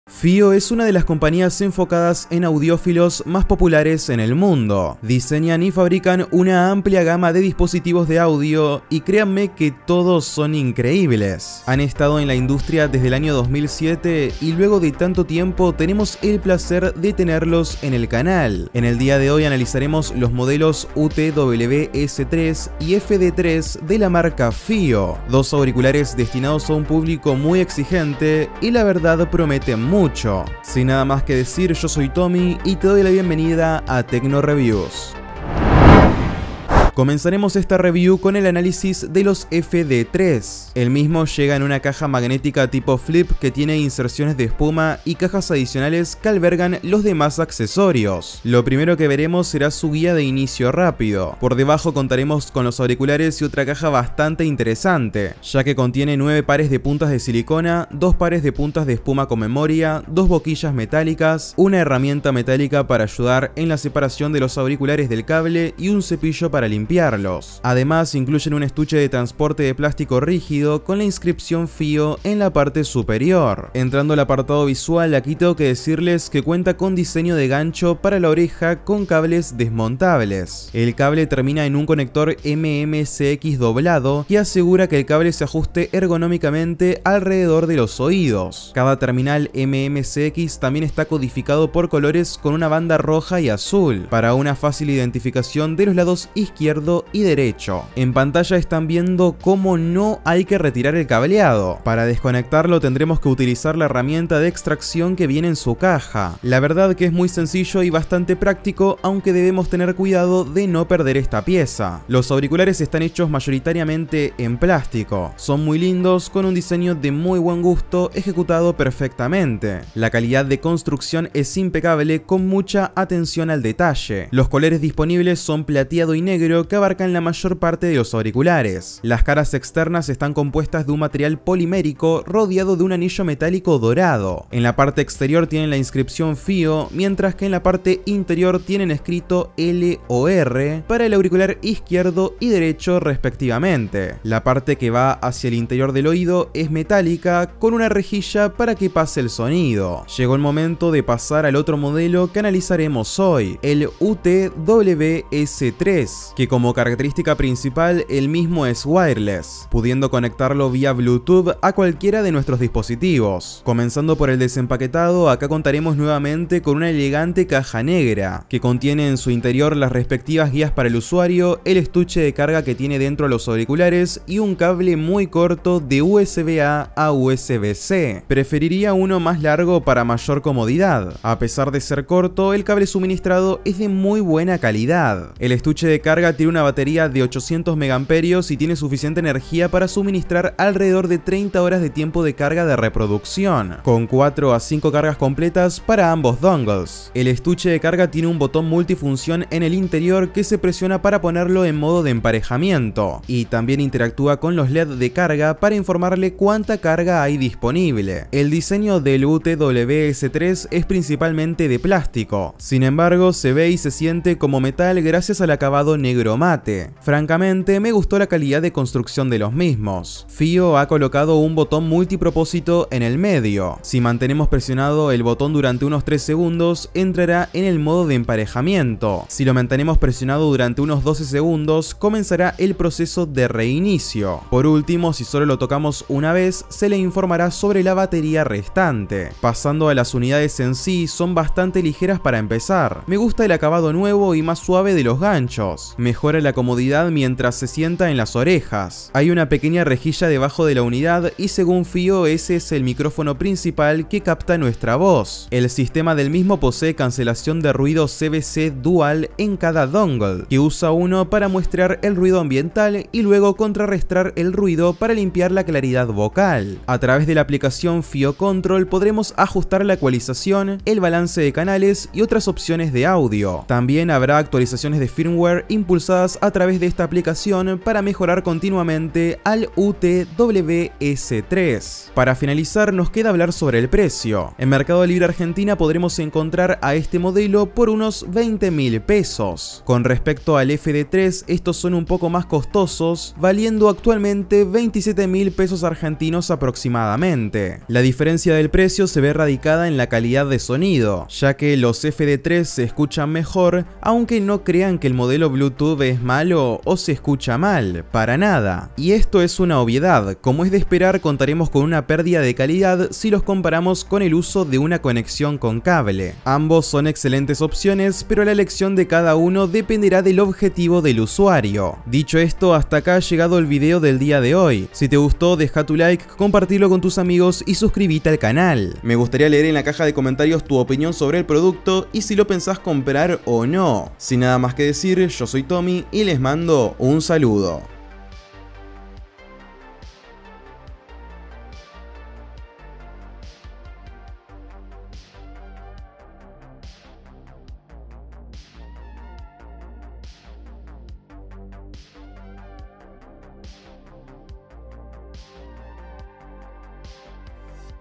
AURICULARES PREMIUM FIIO🤑 ¿VALEN LA PENA🤔?
Fiio utws3 son los amplificadores inalámbricos bluetooth que traemos para analizar junto con los auriculares FD3 de su linea premium💸, ¿Valen la pena👀? acompáñanos a descubrirlo.

⛔SOS DE ARGENTINA? 💸 ARMA O MEJORA TU PC GAMER EN NUESTRA TIENDA 😍👉🏻